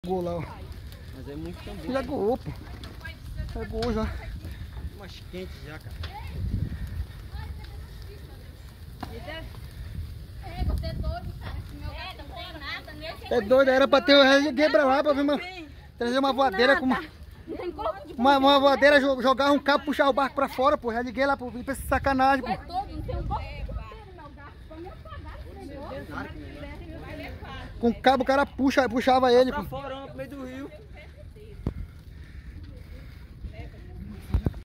Vídeo: barco explode e deixa uma pessoa ferida em Melgaço, no Marajó
Faíscas geradas pela ignição atingiram recipientes com combustível no Pará